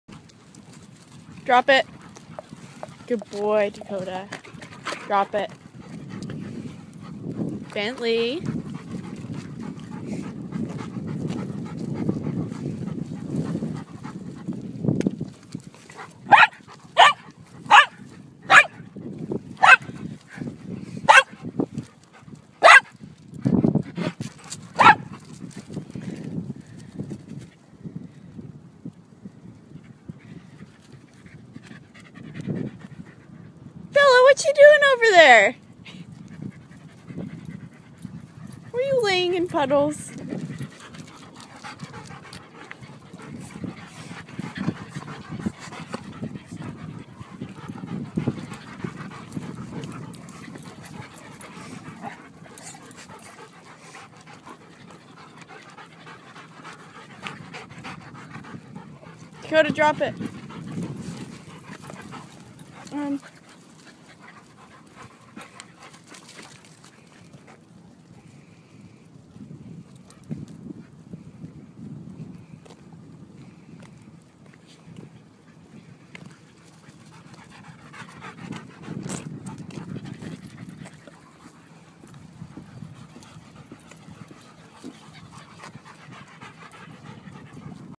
Woofer Walk: Thurs. Afternoon. Feb. 18, 2016
Woofer Walker: Cassy
Woofers:
-Dakota
-Archie
-Max Nunweiler
-Bentley
-Charlie Ayala
-Bella Munn